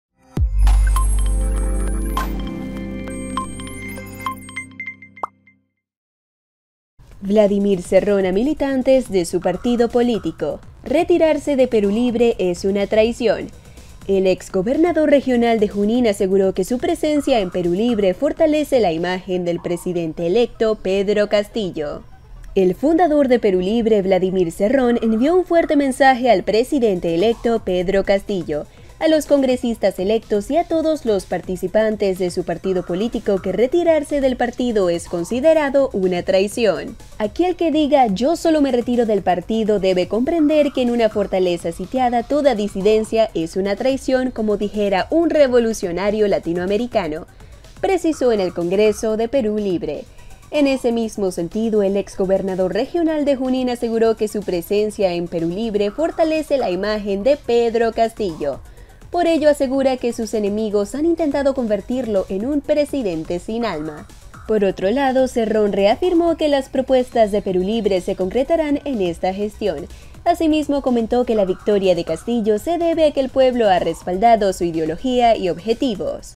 CERRÓN AMEN4ZA A MILITANTES DE SU PARTIDO: "RETIRARSE DE PERÚ LIBRE ES UNA TRAICIÓN"
Vladimir Cerrón a militantes de su partido político: “Retirarse de Perú Libre es una traición”.
El exgobernador regional de Junín aseguró que su presencia en Perú Libre fortalece la imagen del presidente electo, Pedro Castillo.